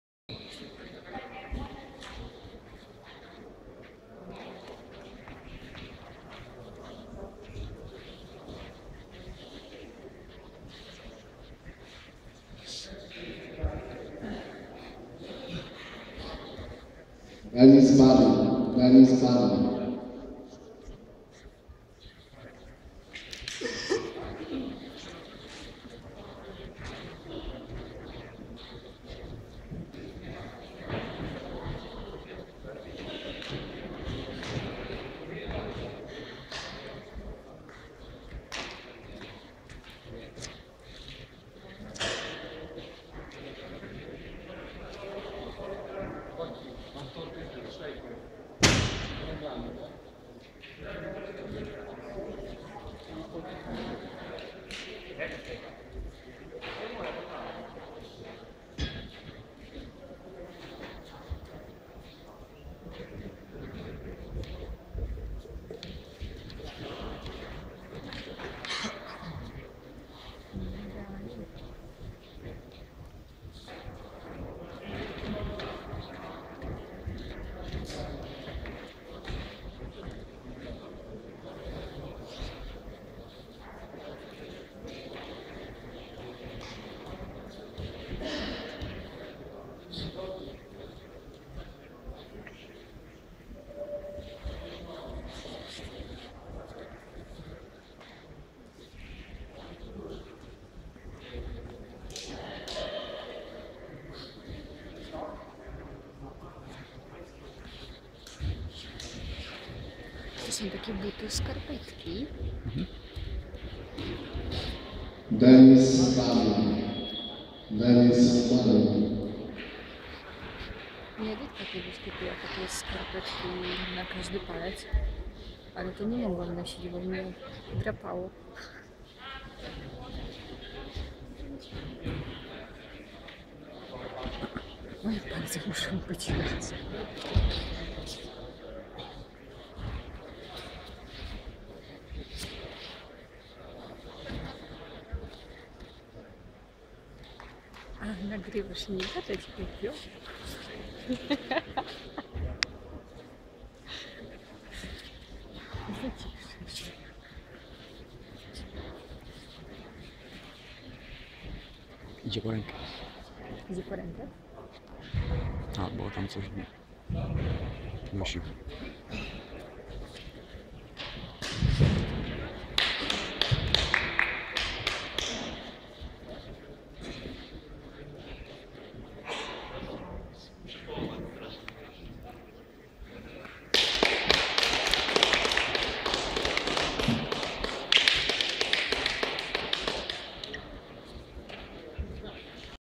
Maksymilian Wiśniewski vs Sebastian Maciejewski, Poland National Pro Jiu-jitsu Championship 2014
Poland National Pro Jiu-Jitsu Championship, Warsaw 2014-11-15
Abu Dhabi World Pro Trials
Brown/Black open